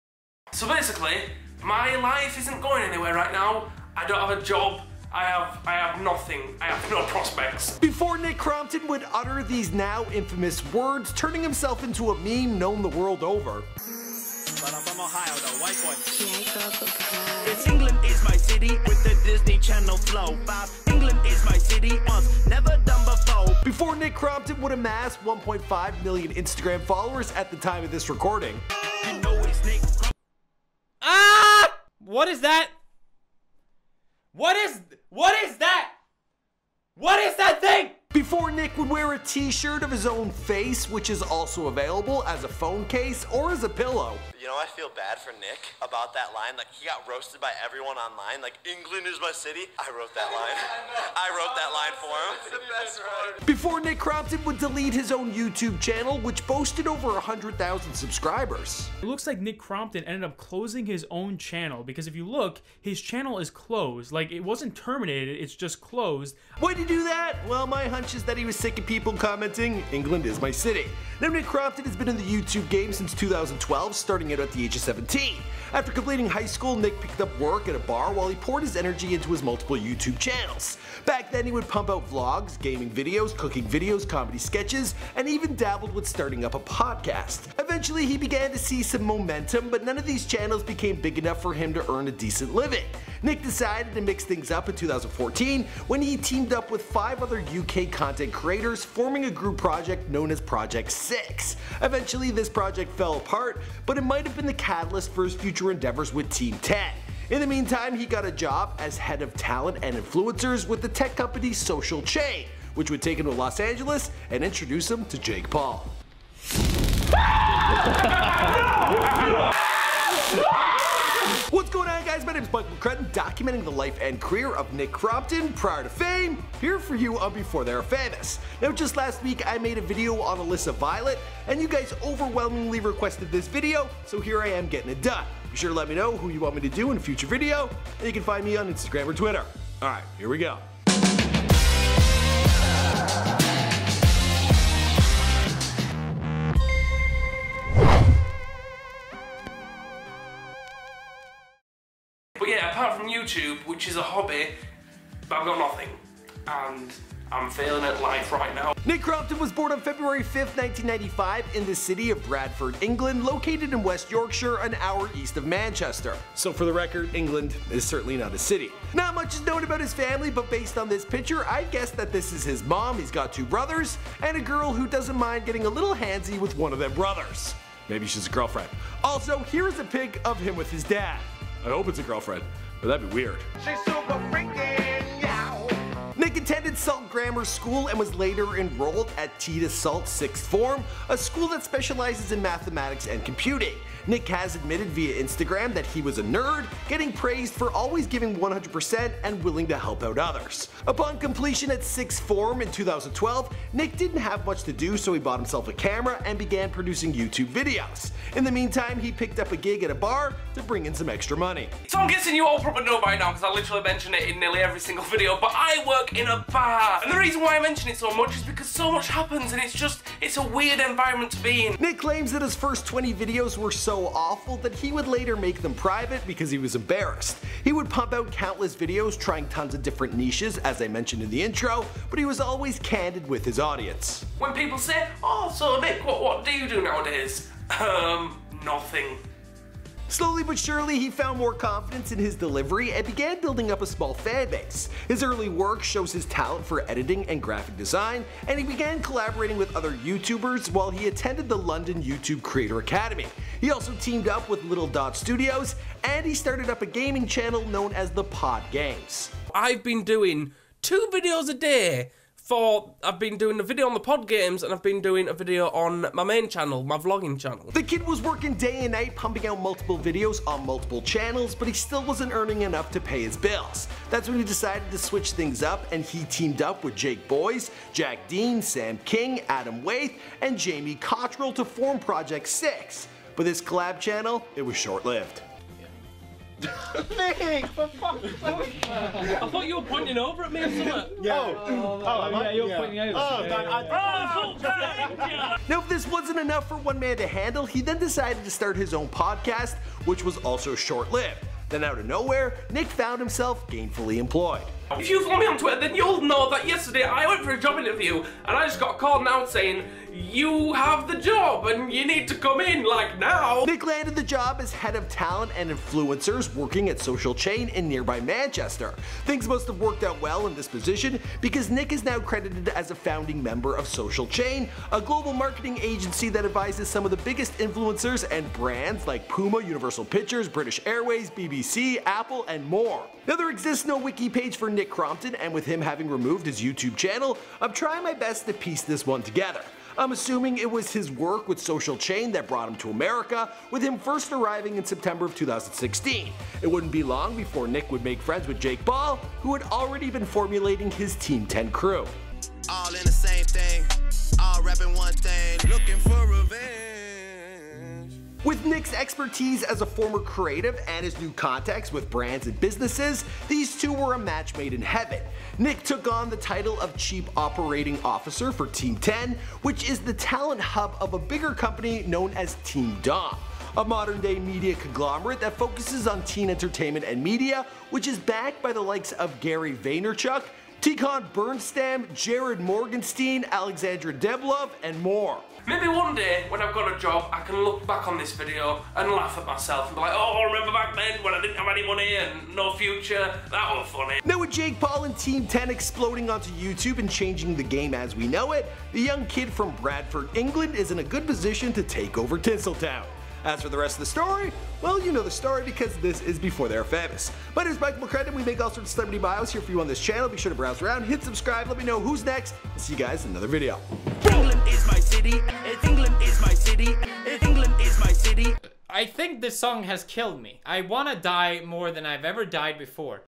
NICK CROMPTON - Before They Were Famous - ENGLAND IS MY CITY
Nick Crompton has been in the YouTube game since 2012 starting out at the age of 17. After completing highschool Nick picked up work at a Bar while he poured all his energy into his multiple YouTube channels. Back then he would pump out vlogs, gaming videos, cooking videos, comedy sketches and even dabbled with starting up a podcast. Find out more in this video: